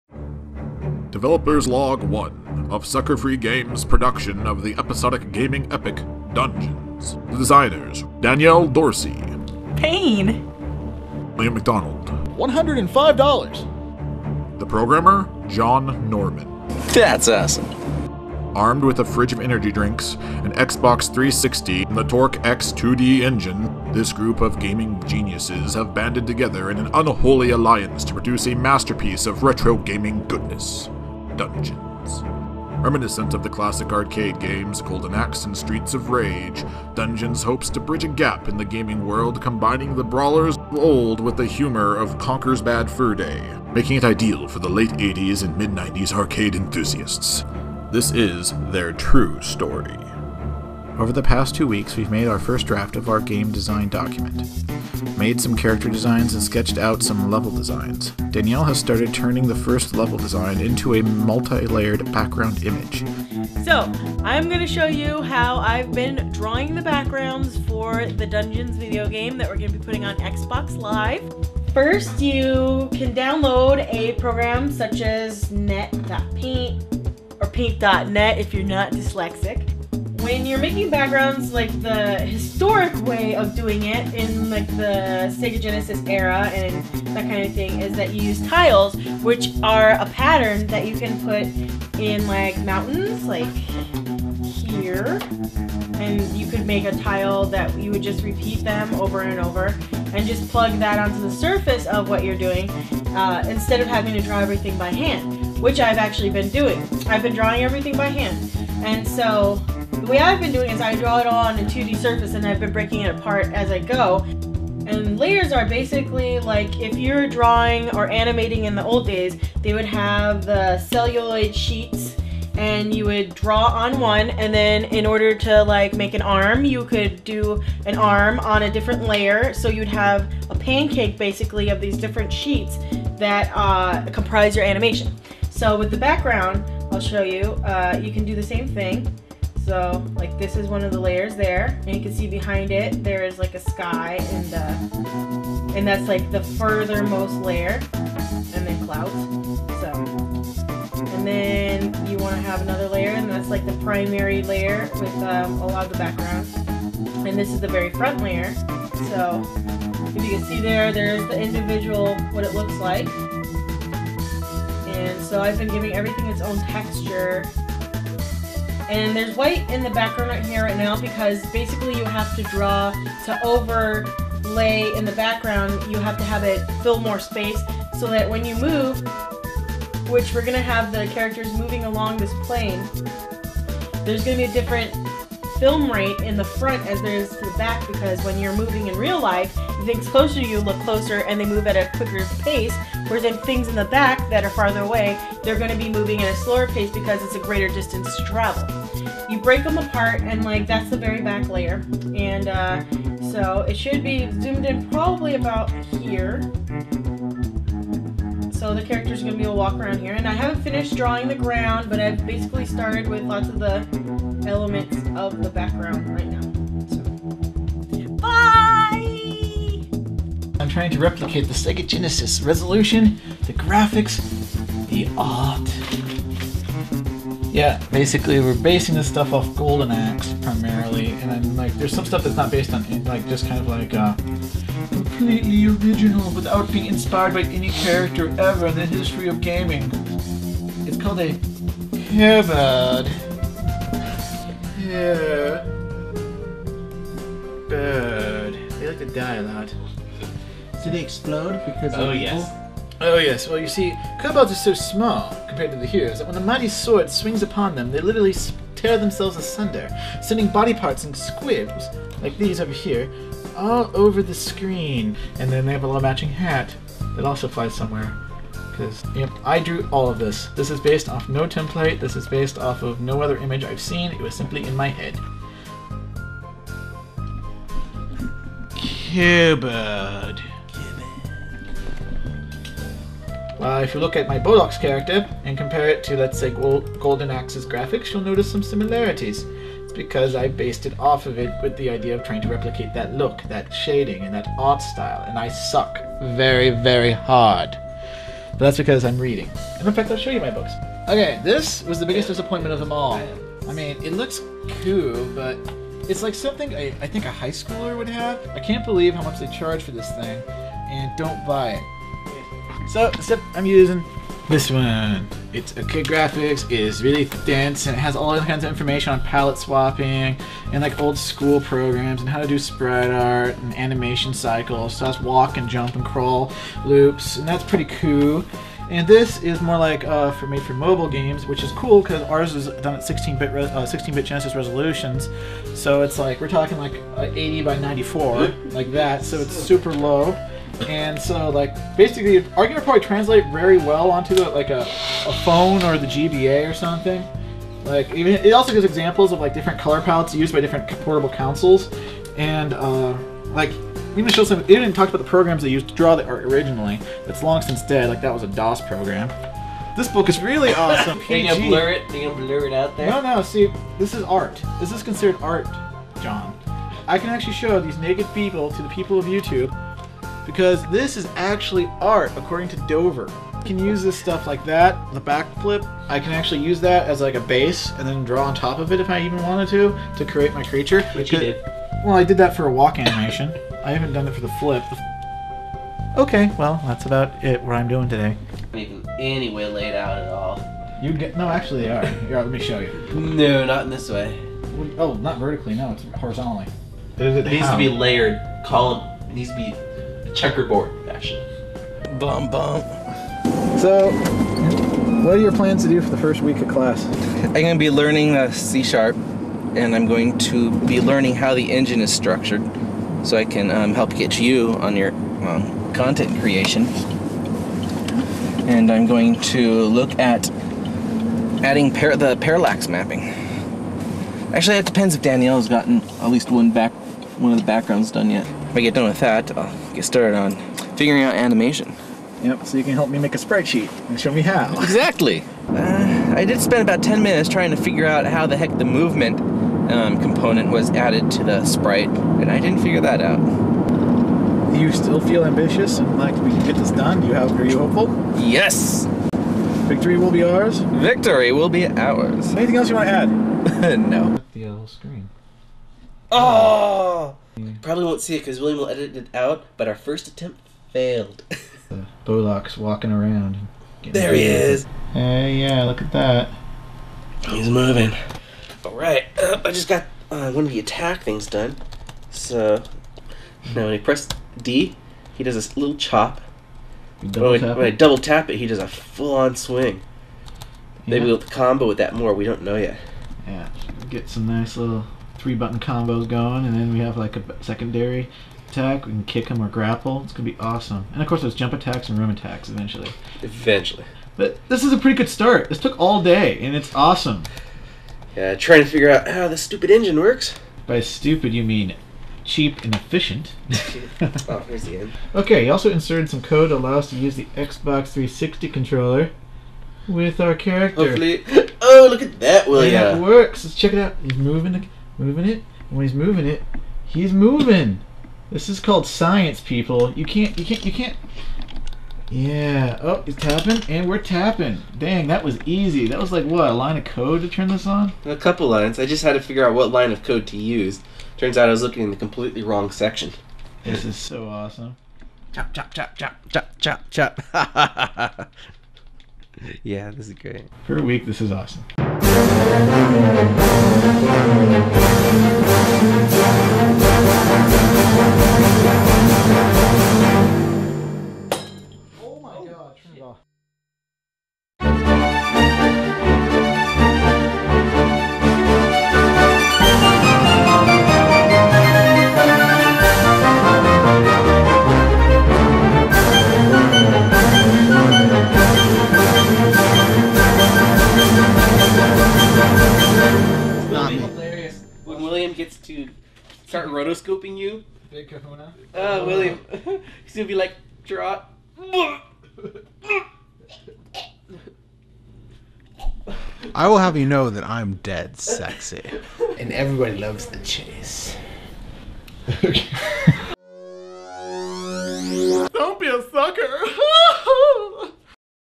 Making a great retro game. Episode 1 XNA Torque and BEYOND!
This is the first of a series of bi-monthly installments cataloging the progress of our video game, "Dungeons" development. When the game is finished it will be available on XBOX Live Community games and we are planning on entering the Dream it Build it competition. Watch us as our game evolves, there are some tutorials and you will get a sneak peak at a game by gamers for gamers. Of the late 80's early 90's. Kobold.